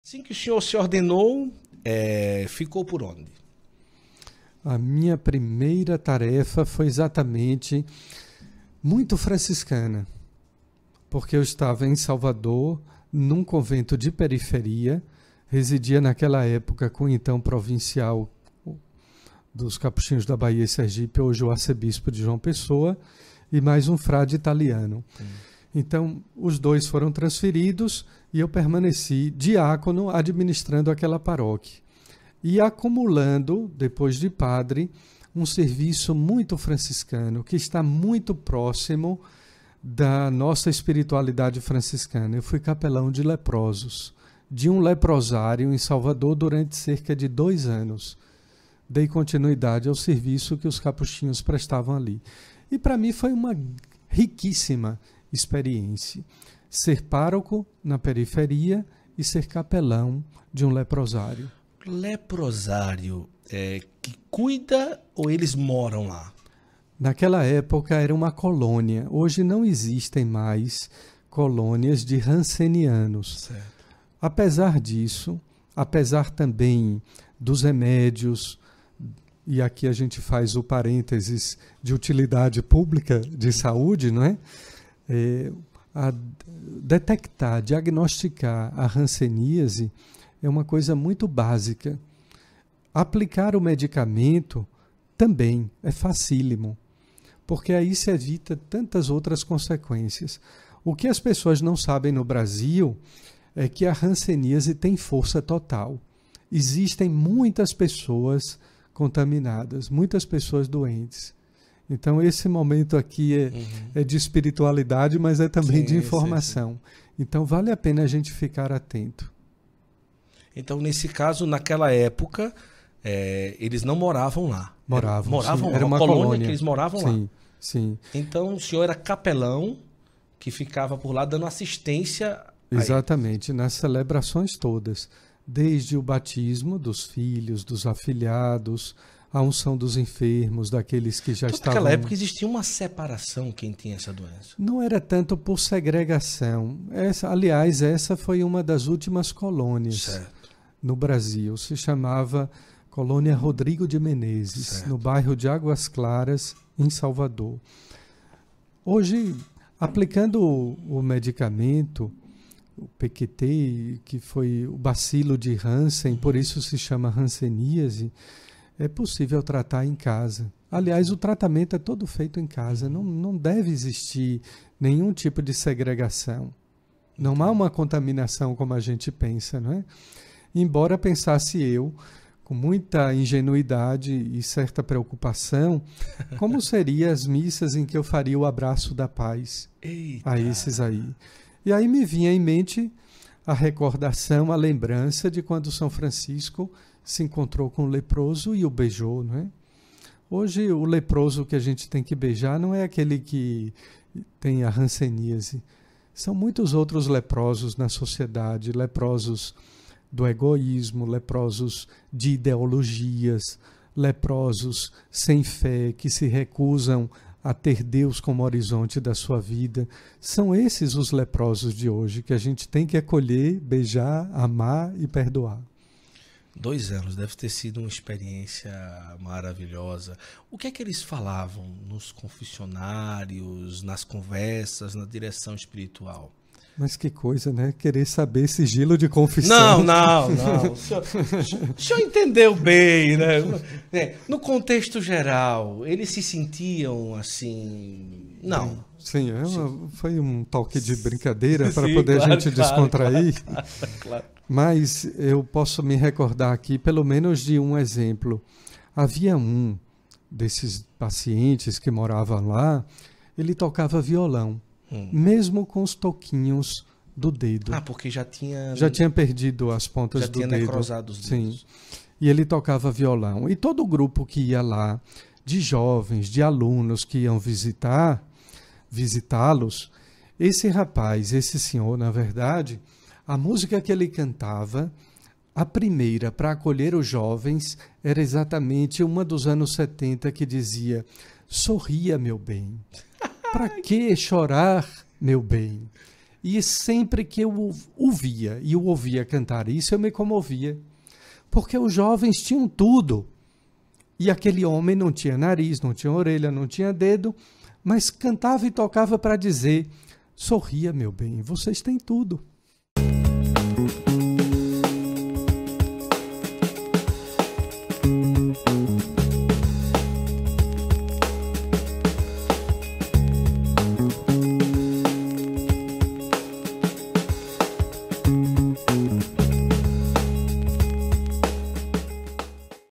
TESTEMUNHO SOBRE LEPROSOS - DOM JOSÉ RUY | Cortes do SantoFlow Podcast
Dom José Ruy é bispo diocesano da Diocese de Caruaru, capuchinho e foi uma grande irmão do saudoso Dom Henrique Soares. Uma conversa cheia de espiritualidade e fortalecimento para nossa fé. Vamos conhecer seu testemunho sobre Dom Henrique, sua história de vida e com muito bom humor!

APRESENTADOR:
Guto Azevedo - Instagram @gutoazevedooficial

CONVIDADO:
Dom José Ruy - Instagram @domjoseruyoficial

CAPTAÇÃO E TRANSMISSÃO:
Studio SantoFlow

REALIZAÇÃO:
Studio SantoFlow